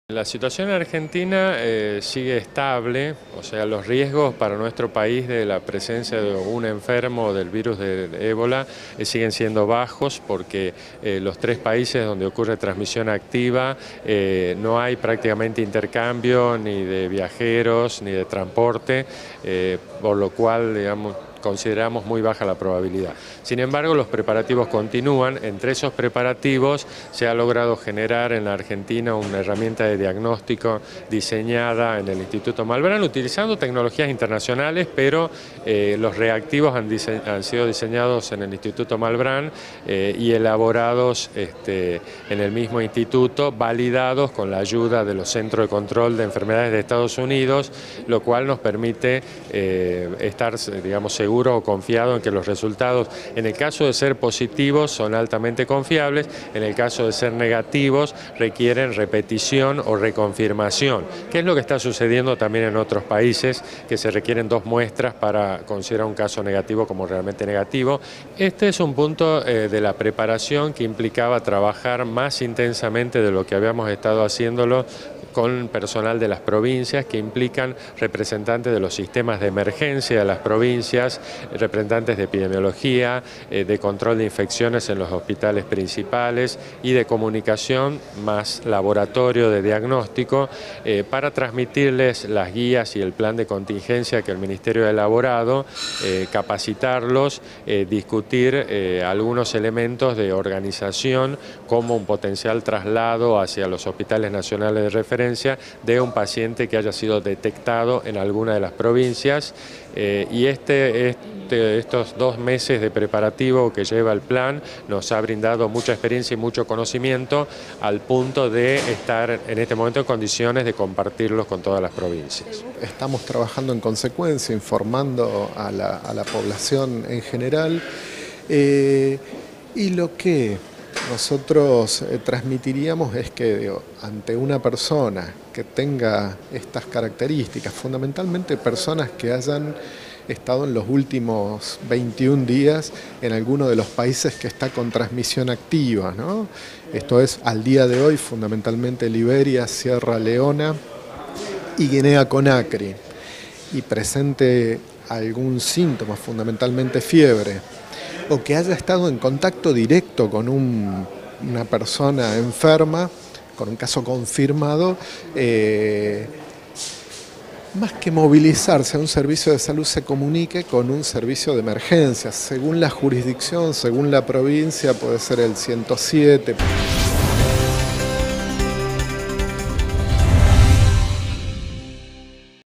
Avanza la preparación en todo el país para responder al ébola
La respuesta de los sistemas de salud de todo el país ante la posibilidad de un caso sospechoso de ébola, en el marco de la preparación del plan de contingencia nacional, fue el tema central de la jornada que reunió hoy a equipos técnicos de las provincias, organizada por la cartera sanitaria nacional, a pesar de que la Argentina es un país de muy bajo riesgo de ingreso del virus.
Representantes de los sistemas de emergencias, de epidemiología, de control de infecciones, de laboratorios de diagnóstico y de comunicación de hospitales provinciales fueron convocados para "transmitirles las guías y el plan de contingencia que el Ministerio de Salud de la Nación elaboró, para capacitarlos y discutir aspectos del plan", aseguró a Télam el viceministro de Salud, Jaime Lazovski.
Destacó que uno de los aspectos a debatir es la organización alrededor de "un potencial traslado hacia los hospitales nacionales de referencia, de un paciente que haya sido detectado en alguna provincia".
Frente a un caso sospechoso de ébola en el interior del país "hay dos situaciones potenciales, una es que esa persona que consulta presenta sólo fiebre y que tengamos margen para realizar un traslado, atención y diagnóstico a nivel nacional".
"Y la otra -continuó- es que el paciente se encuentre en una situación crítica de inicio que dificulte el traslado, en cuyo caso podría ser necesario que permanezca donde se presentó el caso y que ese hospital desarrolle las capacidades de contención y aislamiento para evitar diseminar el virus".
En este sentido, señaló que "como preparativo, todas las provincias han elegido un hospital de referencia para un eventual manejo del caso".
La recomendación de la cartera sanitaria nacional para la evaluación y asistencia ante un caso autorreportado del virus de ébola plantea que la persona comunique al servicio de emergencia y aguarde en su domicilio.
Luego, tanto el servicio de emergencia como la dirección de epidemiología evaluarán si es caso sospechoso y de comfirmarlo, se comunica al hospital designado y el servicio de emergencia junto con personal protegido, traslada al paciente al hospital designado para el aislamiento.
No obstante, Lazovski enfatizó que "no hay ningún alerta para la población argentina, por eso tiene que seguir haciendo sus actividades como siempre; los riesgos para Argentina son muy bajos por la poca vinculación con los países de transmisión activa, que son Guinea, Sierra Leona y Liberia".
Añadió que "los casos que ocurrieron en Estados Unidos, España y Alemania no cambiaron en absoluto la situación de riesgo para Argentina porque en esos países no hay transmisión activa al igual que en Brasil".
"Sin embargo, los preparativos continúan y entre ellos de logró generar en Argentina una herramienta de diagnóstico diseñada en el Instituto Malbrán utilizando tecnología internacional pero los reactivos fueron diseñados y elaborados en el mismo instituto, validados con la ayuda de los centros de control de enfermedades de Estados Unidos", destacó.
El encuentro se realizó en el Hospital El Cruce "Néstor Kirchner", de la localidad bonaerense de Florencio Varela, uno de los tres centros designados y preparados a nivel nacional, junto con los hospitales porteños Garrahan y Militar, para la atención de un eventual caso sospechoso de ébola.
En el marco de avances en los preparativos, el Director de Epidemiología del Ministerio de Salud de la Nación, Juan Herrmann, aseguró que "nos venimos preparando al igual que el resto de los países para esta contingencia"
"Prepararse -precisó- pasa por diferentes instancias, desde revisar los planes de contingencia para este tipo de situación, analizar los circuitos para tener una vigilancia activa y poder identificar la aparición, si bien la probabilidad es baja, de un caso sospechoso en nuestro país".
Dijo asimismo que prepararse pasa por "articular lo que serian las redes de derivación de muestras biológicas; trabajar con la red de servicios de salud, desde el cuidado de una persona sospechosa o confirmada hasta el manejo de los que son los residuos patológicos, coordinar el traslado de una persona enferma de un hospital de menor complejidad a otro de mayor complejidad, con lo cual el plan de contingencia es un plan multidimensional para una situación en la cual en Argentina como en la mayoría de los países no se tiene experiencia, lo cual es un desafío".